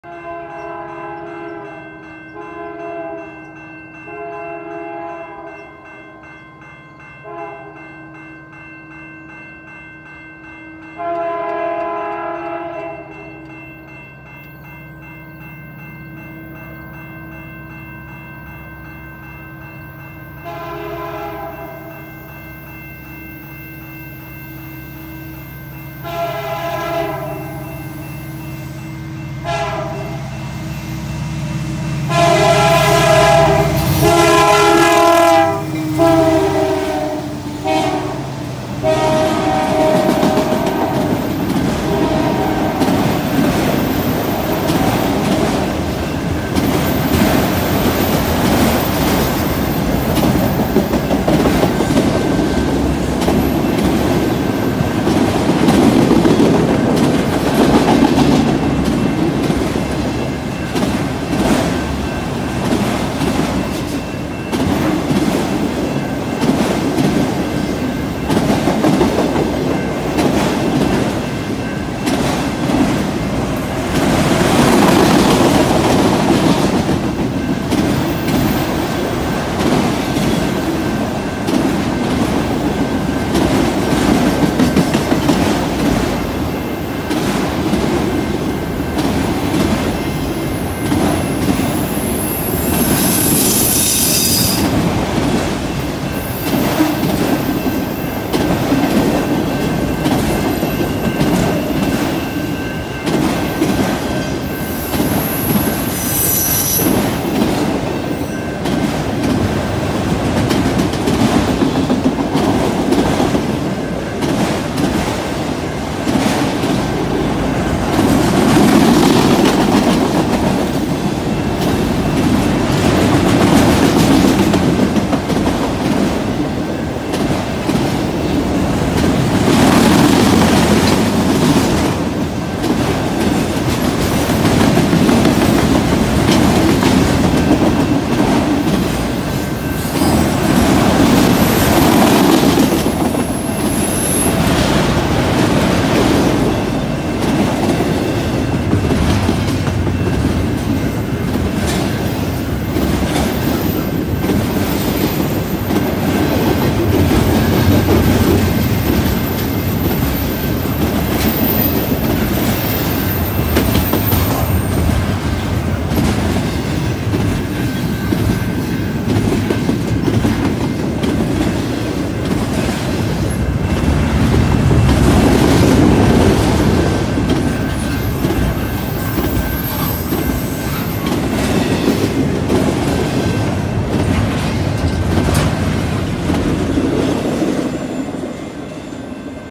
9/16/23 CN Train Q 116
9/16/23 10:02 56 degrees at CN MP 202.1 Charlotte as CN 5713 and CN 2204 power Train Q 116 across Stoddard Street. Full 3:09 video.